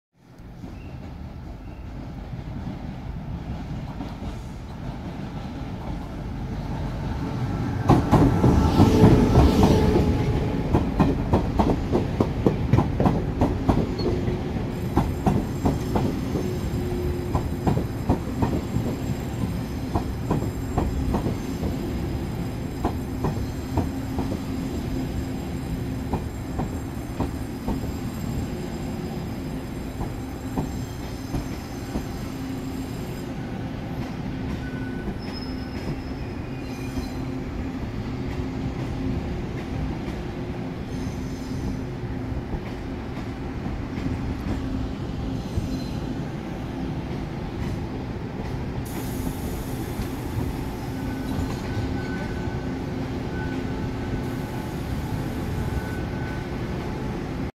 2018/05/19 125次自強號 彰化進站
註:  七堵→屏東(山線)

停靠站:  七堵→汐止→松山→臺北→板橋→樹林→桃園→中壢→新竹→竹南→苗栗→豐原→臺中→彰化→員林→斗六→嘉義→新營→臺南→新左營→高雄→鳳山→九曲堂→屏東